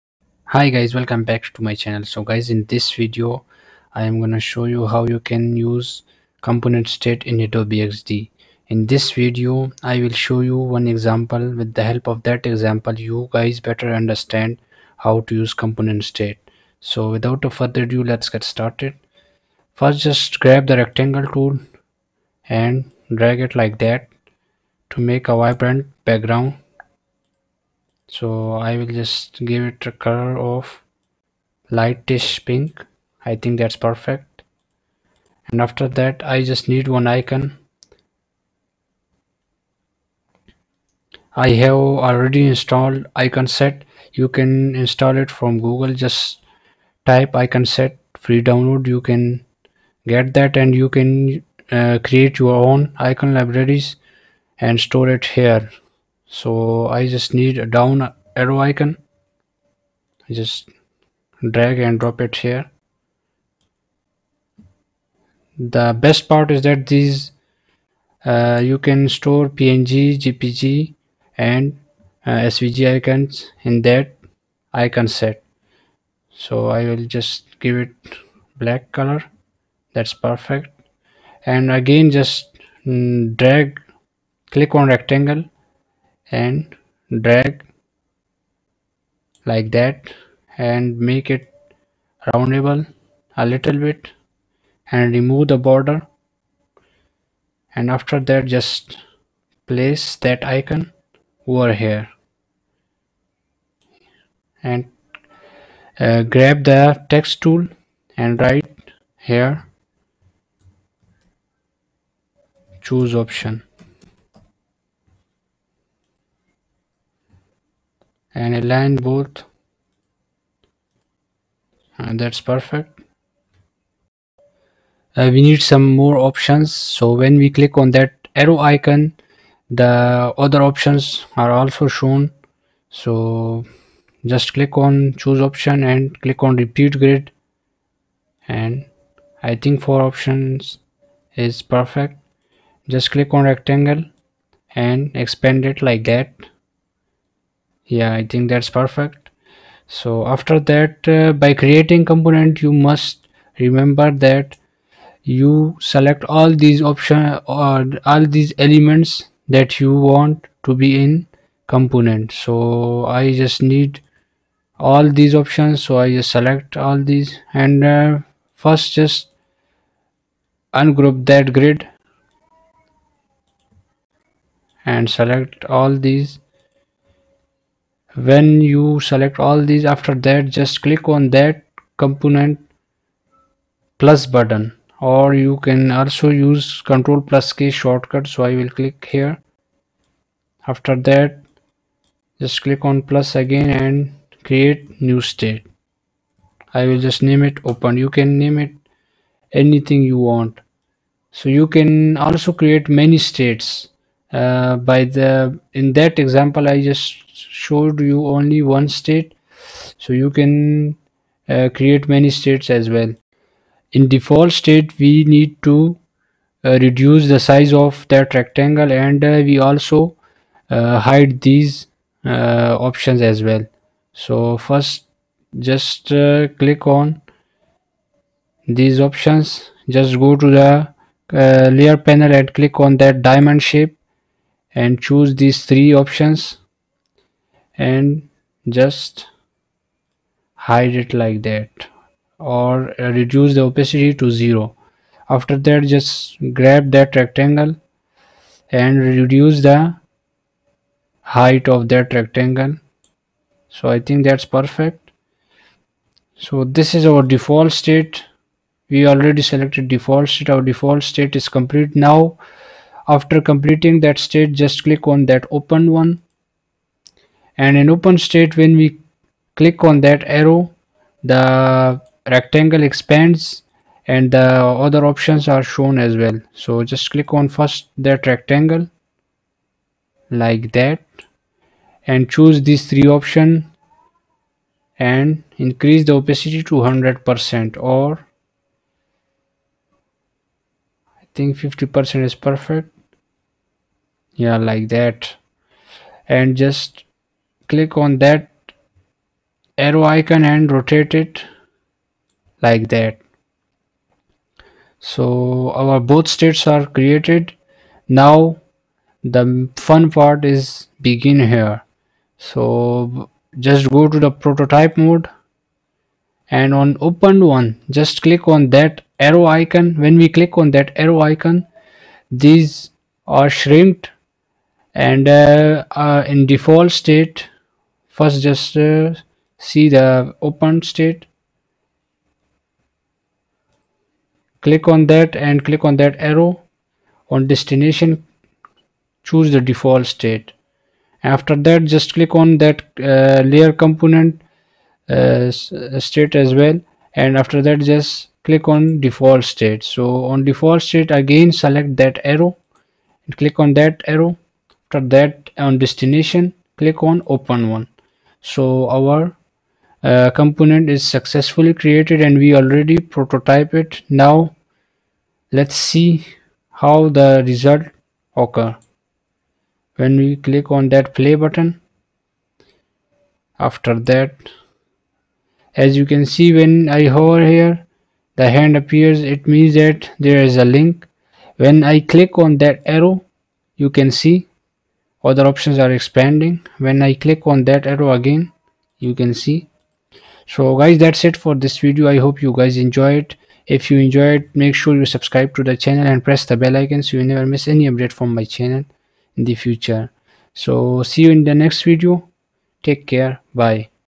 How to use components states in Adobe XD | How to create options menu animation in Adobe XD
In this video I will teach you how you can use component states in adobe xd make sure you watch the complete video because after watching this video you will never forget that concept again. Hope this helps.